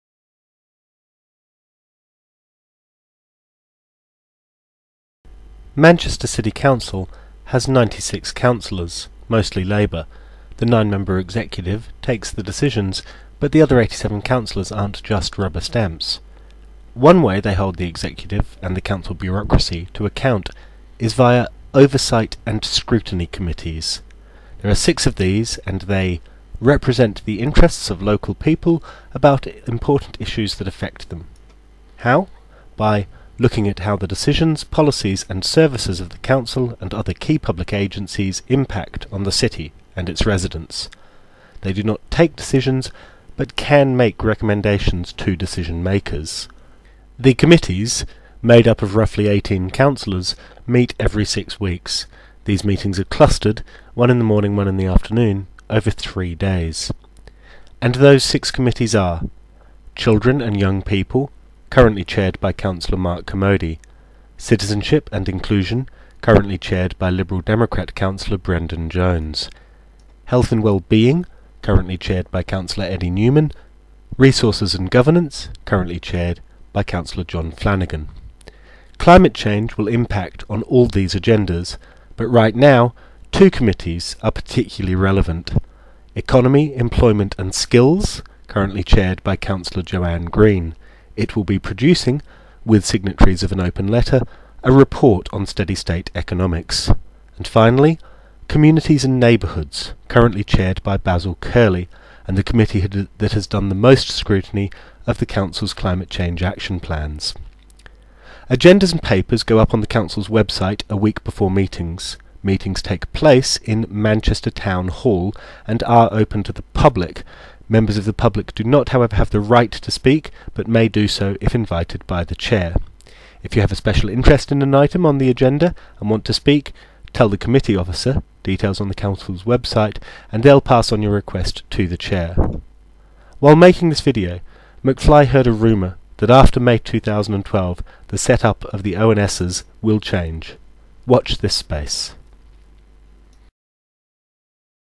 Bluffer's guide to oversight and scrutiny committees
2 and a half minutes of explanation about the why where what and who of the 6 "Oversight and Scrutiny" committees of Manchester City Council, with a special emphasis on the climate agenda.